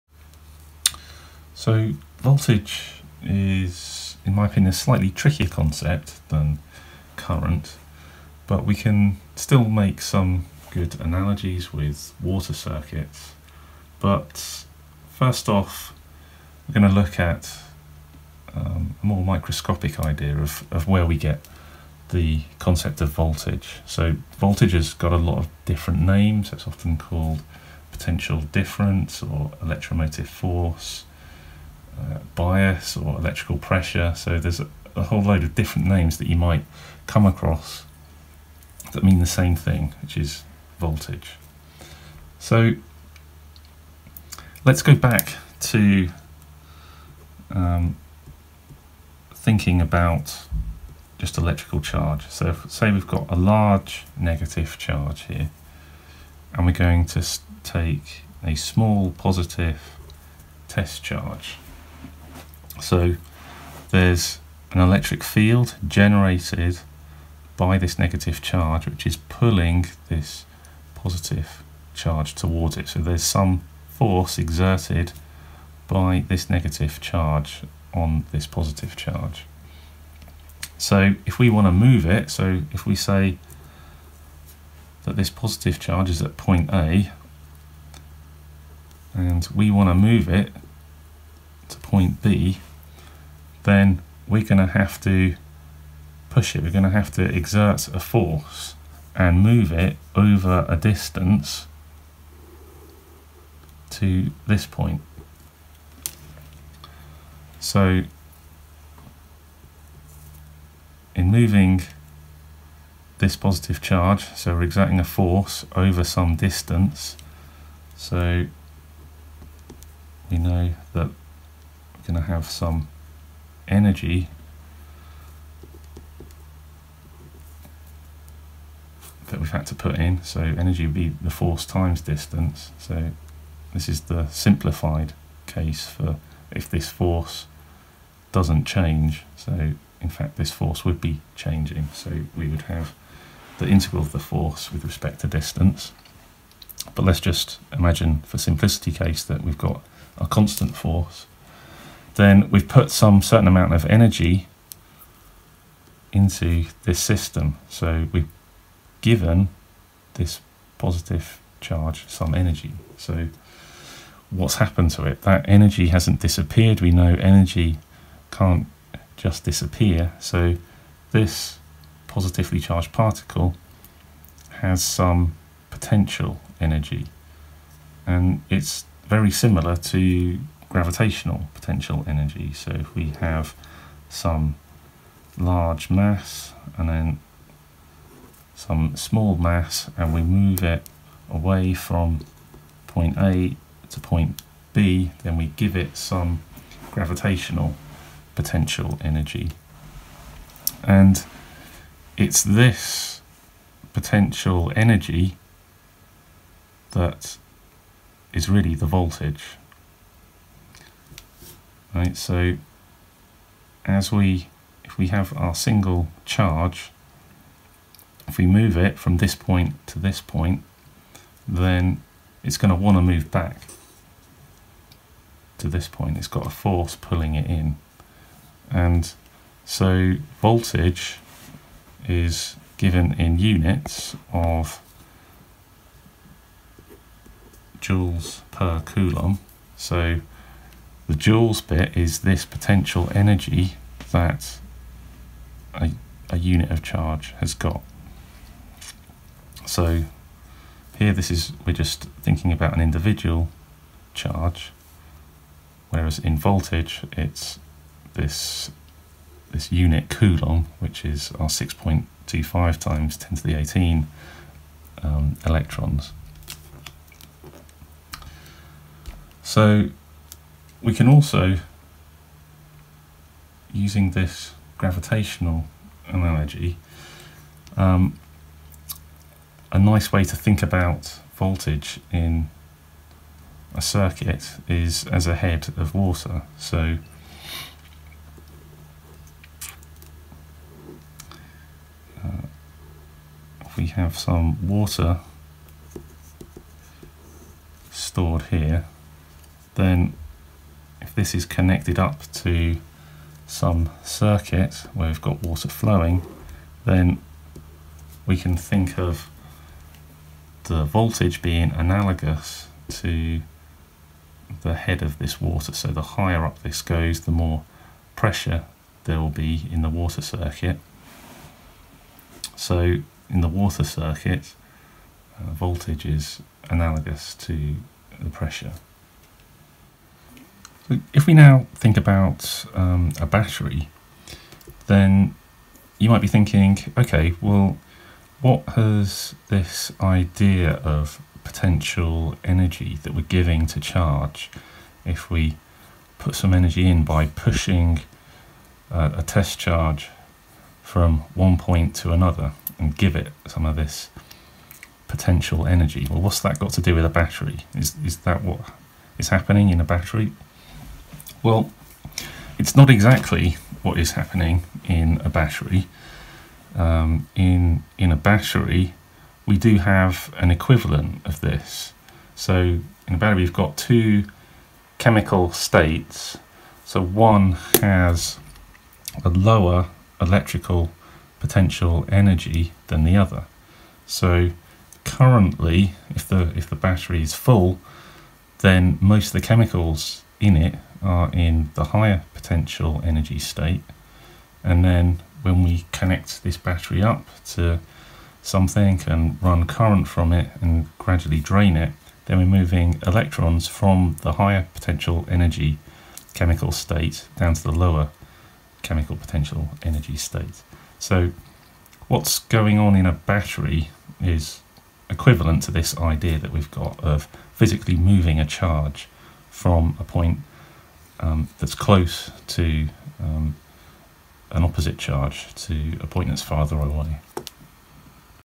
Voltage explained
An explanation of what the concept of voltage in electric circuits is using analogies to water circuits and gravitation.